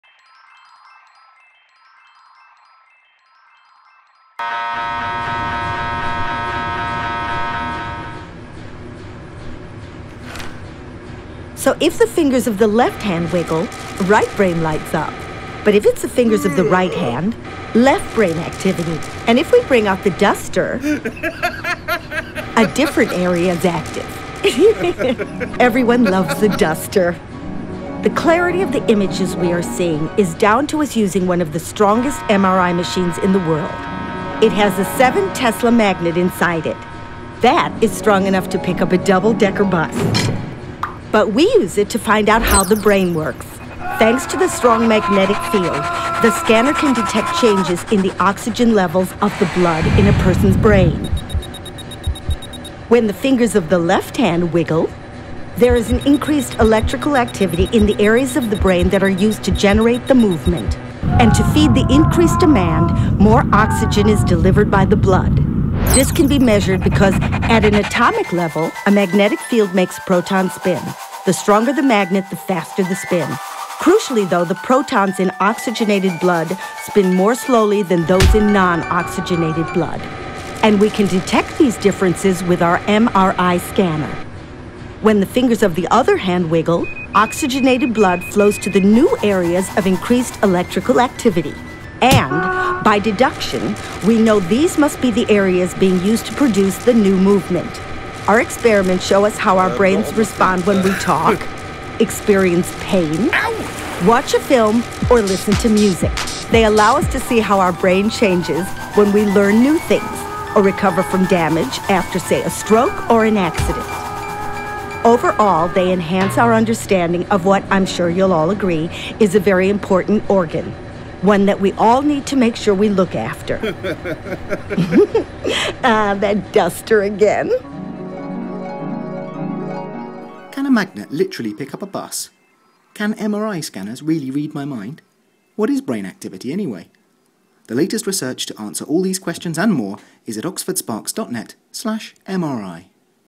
Oxford Sparks: A spin around the brain
Take a journey around the brain with Ossie from Oxford Sparks. Find out more and read about the science behind the animation